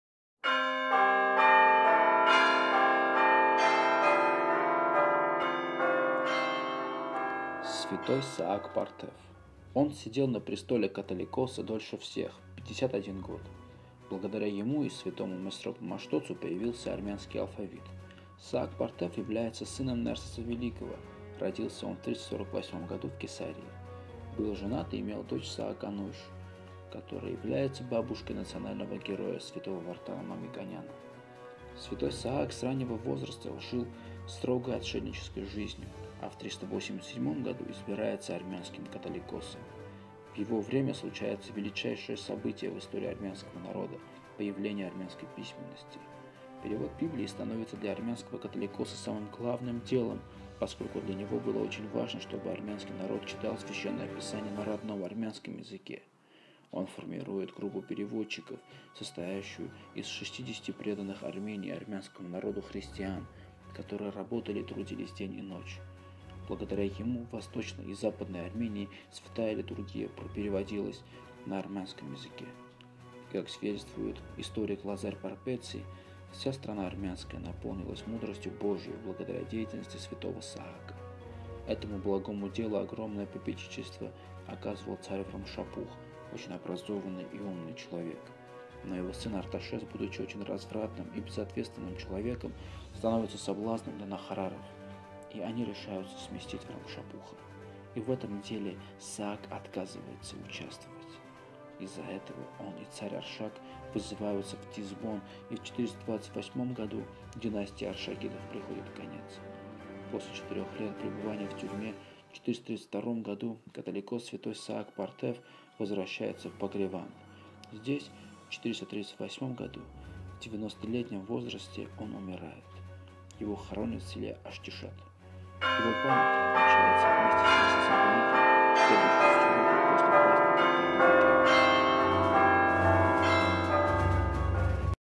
Святой Саак Партев
Переводом на карту Сбербанка: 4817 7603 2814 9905 Святой Саак Партев

Он сидел на престоле католикоса дольше всех: 51 год. В его дни появился армянский алфавит с его активным участием.
Саак Партев является сыном Нерсеса Великого, родился в 348 году в Кесарии. Был женат и имел дочь Саакануйш, которая является бабушкой национального героя святого Вардана Мамиконяна.
Святой Саак с раннего возраста жил строгой отшельнической жизнью, а в 387 году избирается армянским католикосом. В его время случается величайшее событие в истории Армянского народа - армянская письменность.
Перевод Библии становится для армянского Католикоса самым главным делом, поскольку для него было очень важно, чтобы армянский народ читал Священное Писание на родном армянском языке.
Он формирует группу переводчиков, состоящую из 60 преданных Армении и армянскому народу христиан, которые работали и трудились день и ночь.
Благодаря ему в Восточной и Западной Армении святая Литургия проводились на армянском языке. И как свидетельствует Лазарь Парпеци:
Вся страна армянская наполнилось мудростью Божьей благодаря деятельности святого Саака."
Этому благому делу огромное попечительство оказывал царь Врамшапух, очень  образованный и умный человек. Но его сын Арташес будучи очень развратным и безответственным человеком становится соблазном для нахараров, и они решаются сместить Врамшапуха. И в этом деле Саак отказывается участовать. Из за этого он и царь Аршак вызываются в Тизбон и в 428 году династии Аршакидов приходит конец. После 4 лет пребывания в тюрьме 432 году католикос святой Саак Партев возвращается в Багреванд. Здесь в 438 году в 90 летнем возрасте он умирает. Его хоронят в селе Аштишат.
Его память отмечается вместе с Нерсесом Великим в следующую субботу после праздника Катохике.